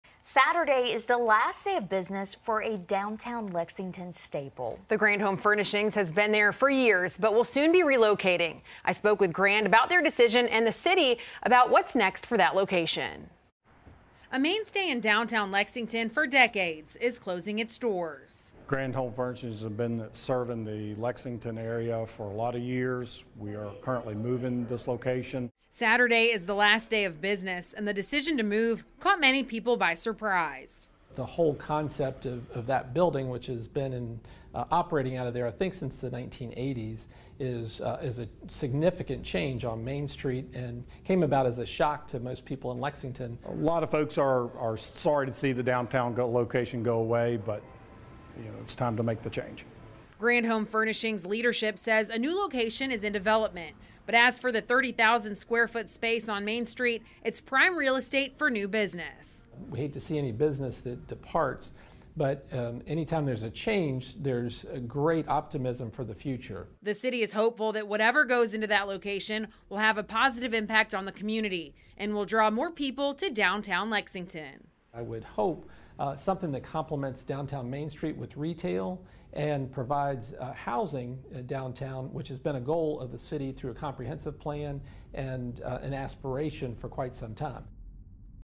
Grand Home Furnishings in Downtown Lexington closes
Saturday is the last day of business for downtown Lexington staple.